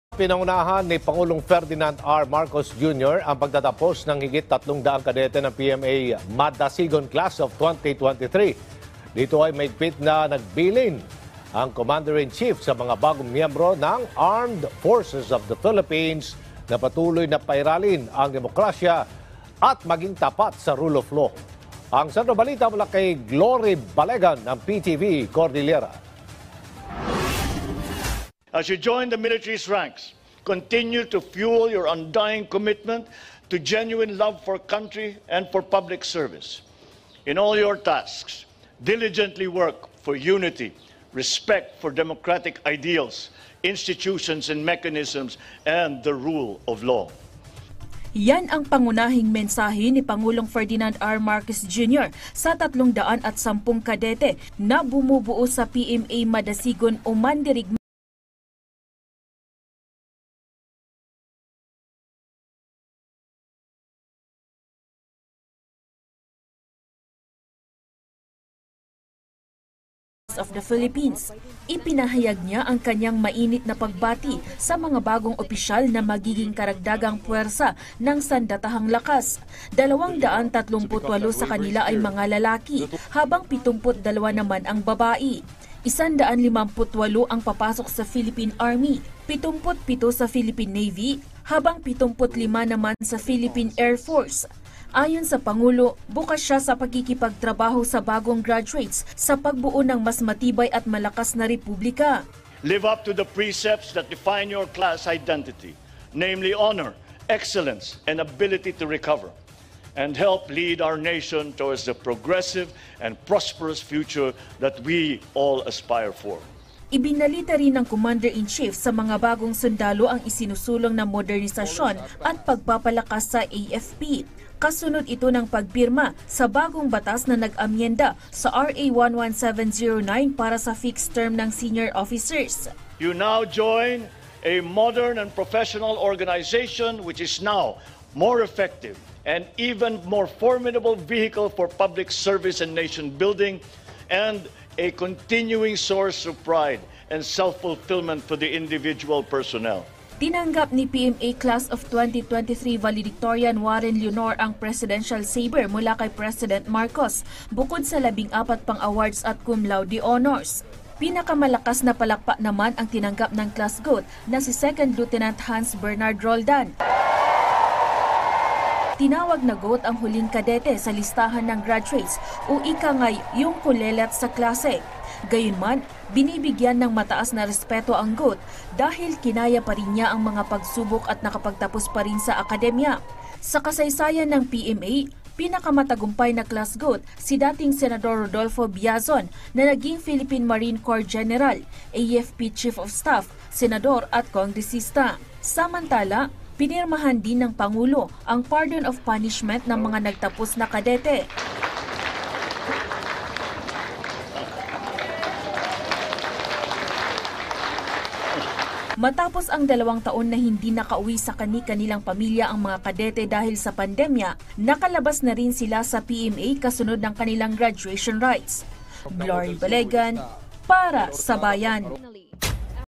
PBBM, pinangunahan ang graduation rites ng PMA Madasigon Class of 2023
PBBM, pinangunahan ang graduation rites ng PMA Madasigon Class of 2023;  patuloy na modernisasyon at pagpapatibay sa hanay ng militar, tiniyak ng Pangulo